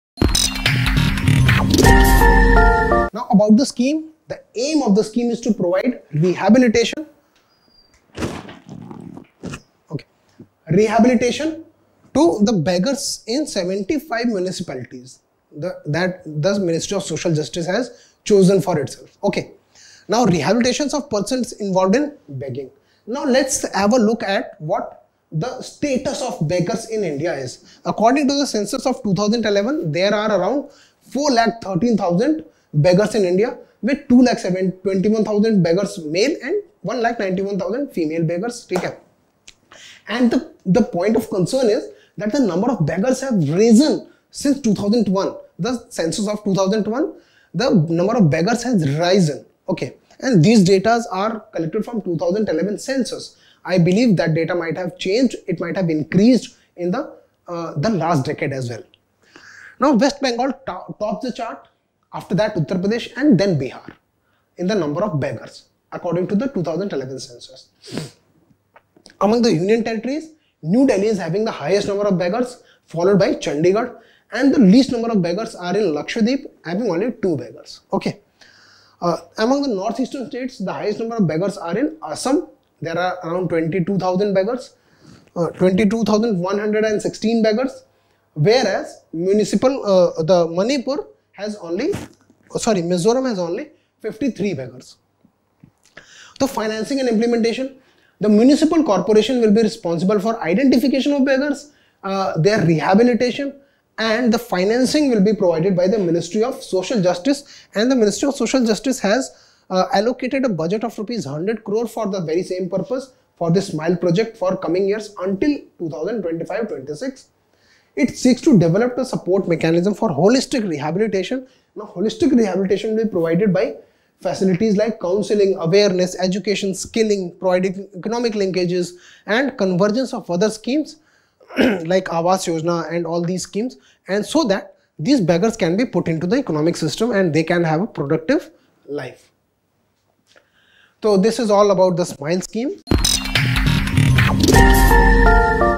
SMILE 75 Initiative | Current Affairs | Amit Tiwari | Tarun IAS
Live Streaming | Watch Online Current Affairs  Strategy for UPSC CSE by Amit Sir. This Video is based on the Current Affairs Strategy for UPSC CSE by Amit Sir on the behalf of Tarun IAS.